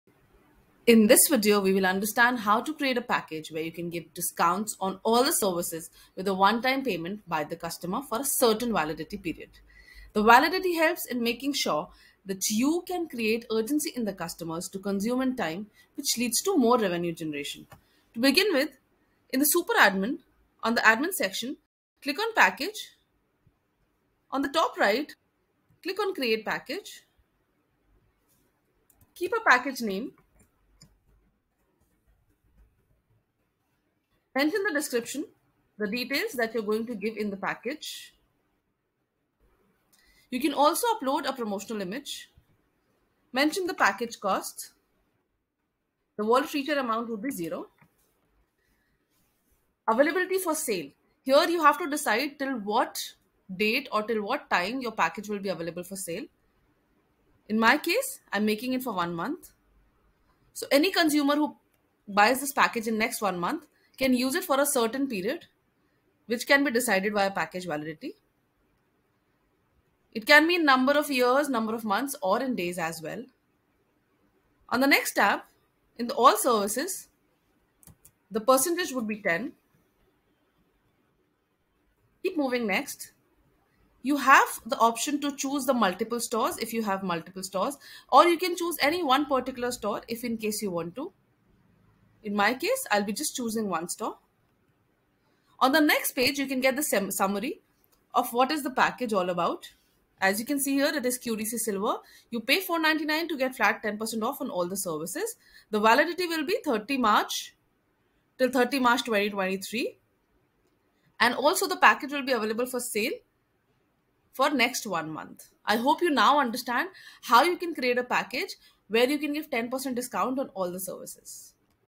Packages: Discount based packages- QDC Silver
In this video, we will understand how to create a package where you can give discounts on all the services with one-time payment by the customer for a certain validity period. The validity helps in making sure that you can create urgency in the customers to consume in time which leads to more revenue generation.

-How to create a service discount based package
-How to select the Package amount along with package validity